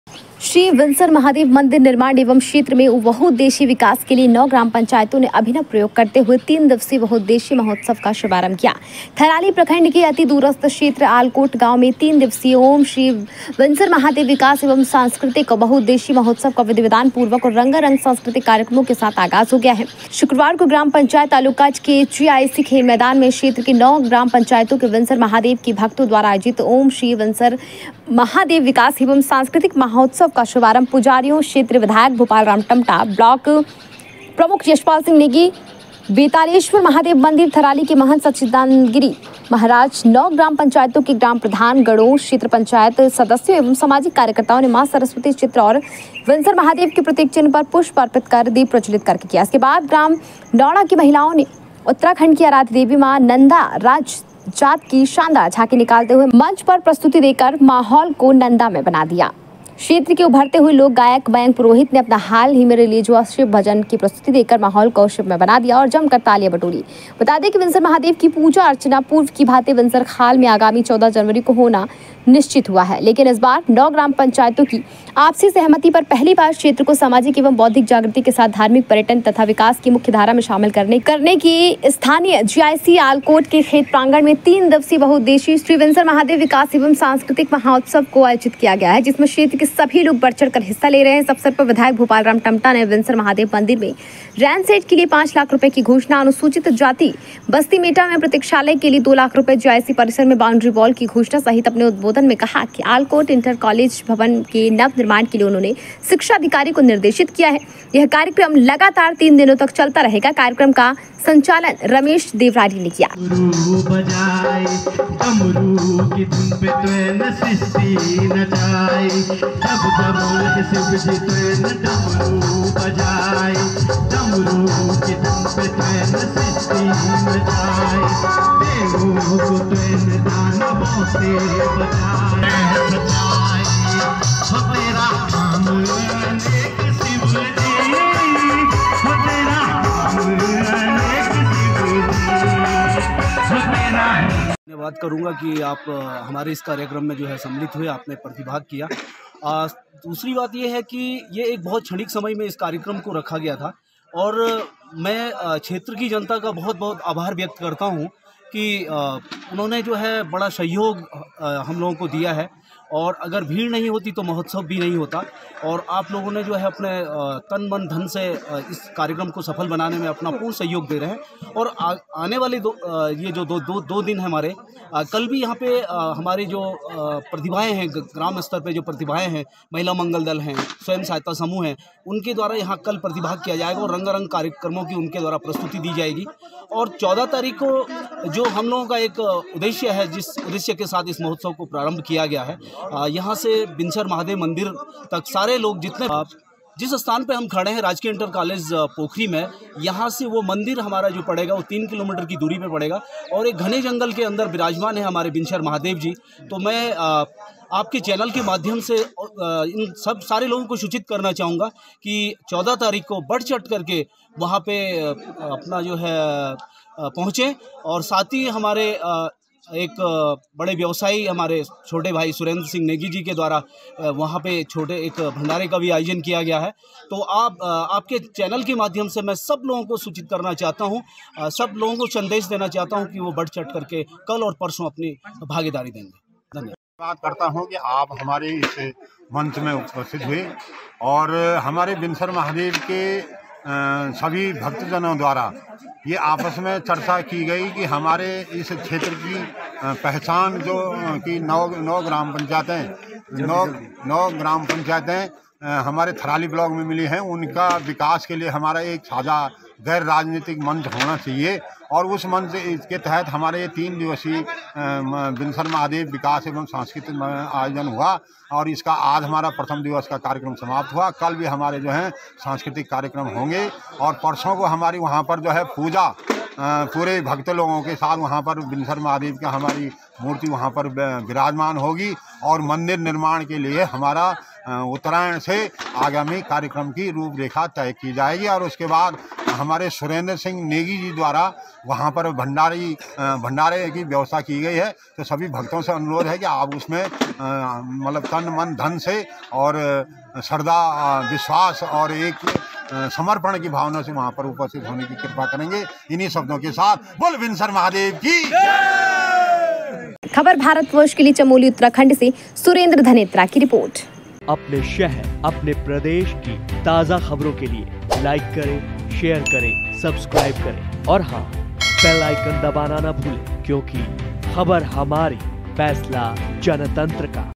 चमोली : श्री विन्सर महादेव मंदिर निर्माण एवं क्षेत्र में विकास के लिए 3 दिवसीय महोत्सव का शुभारंभ
चमोली (उत्तराखंड) : श्री विन्सर महादेव मंदिर निर्माण एवं क्षेत्र में विकास के लिए 3 दिवसीय महोत्सव का शुभारंभ